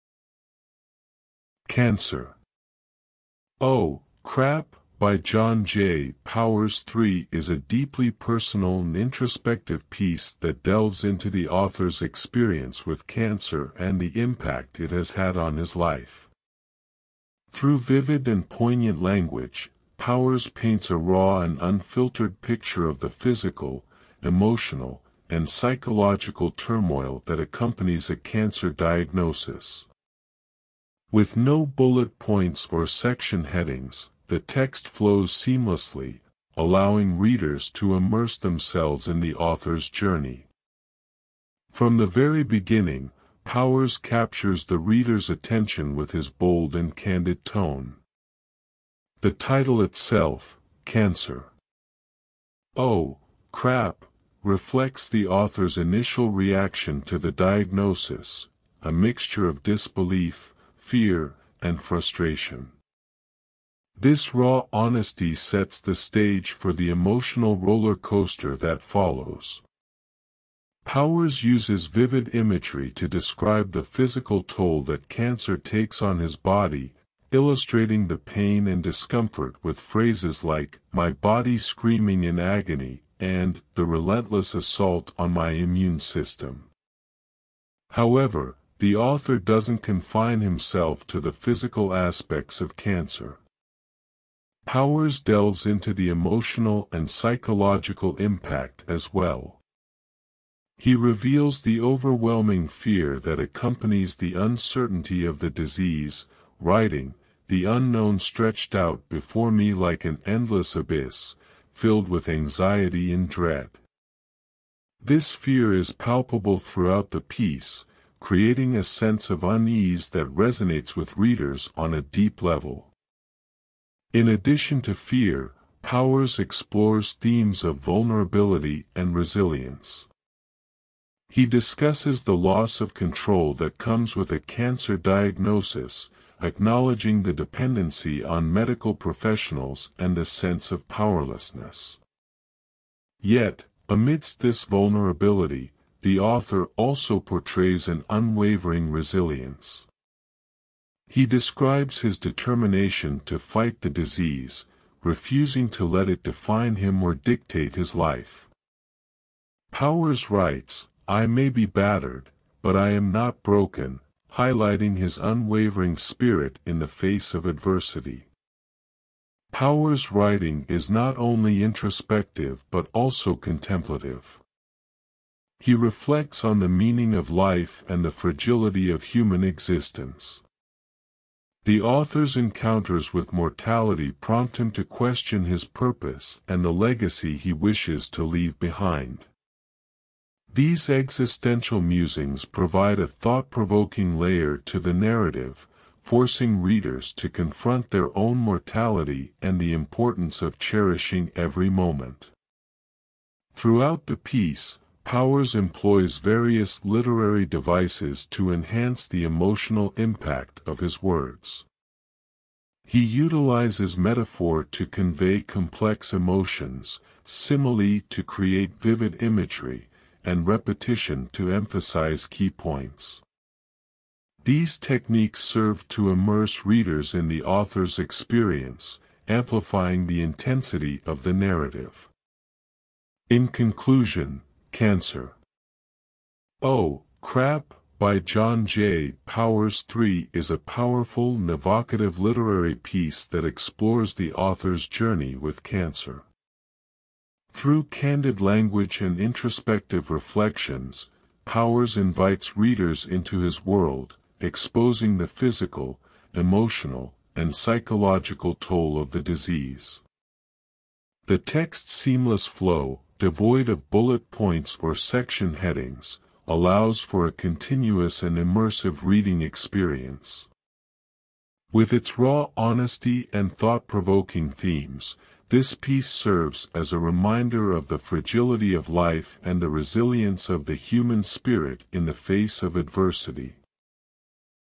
"Cancer. Oh, Crap." By John J. Powers III
"Cancer. Oh, Crap." by John J. Powers III is a deeply personal and introspective piece that delves into the author's experience with cancer and the impact it has had on his life. Through vivid and poignant language, Powers paints a raw and unfiltered picture of the physical, emotional, and psychological turmoil that accompanies a cancer diagnosis. With no bullet points or section headings, the text flows seamlessly, allowing readers to immerse themselves in the author's journey.From the very beginning, Powers captures the reader's attention with his bold and candid tone. The title itself, "Cancer. Oh, Crap.," reflects the author's initial reaction to the diagnosis—a mixture of disbelief, fear, and frustration. This raw honesty sets the stage for the emotional rollercoaster that follows. Powers uses vivid imagery to describe the physical toll that cancer takes on his body, illustrating the pain and discomfort with phrases like "my body screaming in agony" and "the relentless assault on my immune system."However, the author doesn't confine himself to the physical aspects of cancer. Powers delves into the emotional and psychological impact as well. He reveals the overwhelming fear that accompanies the uncertainty of the disease, writing, "The unknown stretched out before me like an endless abyss, filled with anxiety and dread." This fear is palpable throughout the piece, creating a sense of unease that resonates with readers on a deep level.In addition to fear, Powers explores themes of vulnerability and resilience. He discusses the loss of control that comes with a cancer diagnosis, acknowledging the dependency on medical professionals and the sense of powerlessness. Yet, amidst this vulnerability, the author also portrays an unwavering resilience. He describes his determination to fight the disease, refusing to let it define him or dictate his life. Powers writes, "I may be battered, but I am not broken," highlighting his unwavering spirit in the face of adversity.Powers' writing is not only introspective but also contemplative. He reflects on the meaning of life and the fragility of human existence. The author's encounters with mortality prompt him to question his purpose and the legacy he wishes to leave behind. These existential musings provide a thought-provoking layer to the narrative, forcing readers to confront their own mortality and the importance of cherishing every moment.Throughout the piece, Powers employs various literary devices to enhance the emotional impact of his words. He utilizes metaphor to convey complex emotions, simile to create vivid imagery, and repetition to emphasize key points. These techniques serve to immerse readers in the author's experience, amplifying the intensity of the narrative.In conclusion, "Cancer. Oh, Crap." by John J. Powers III is a powerful and evocative literary piece that explores the author's journey with cancer. Through candid language and introspective reflections, Powers invites readers into his world, exposing the physical, emotional, and psychological toll of the disease. The text's seamless flow, devoid of bullet points or section headings, allows for a continuous and immersive reading experience. With its raw honesty and thought-provoking themes, this piece serves as a reminder of the fragility of life and the resilience of the human spirit in the face of adversity.